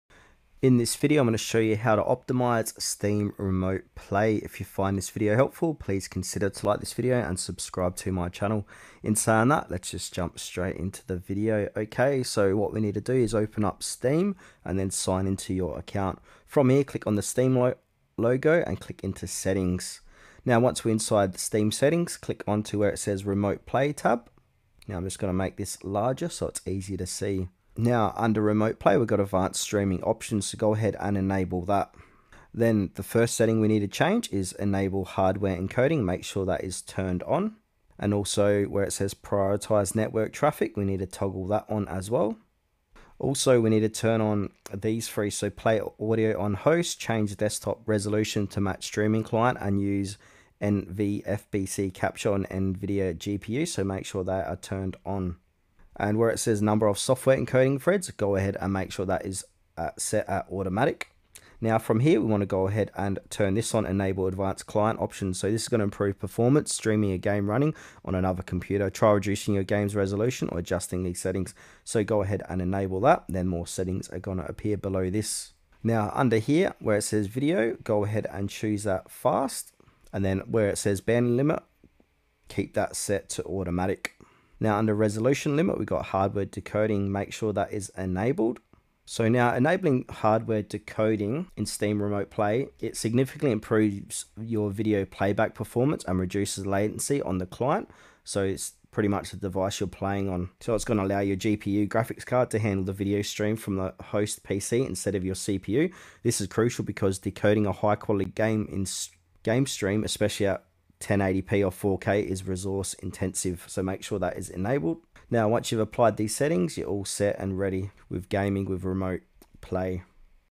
How To Optimize Steam Remote Play
In this quick tutorial, you'll learn how to optimize Steam Remote Play for the best streaming experience. Whether you want smoother gameplay or reduced lag, these simple tips and settings adjustments will help you get the most out of remote gaming. Perfect for enjoying your favorite games from anywhere with ease!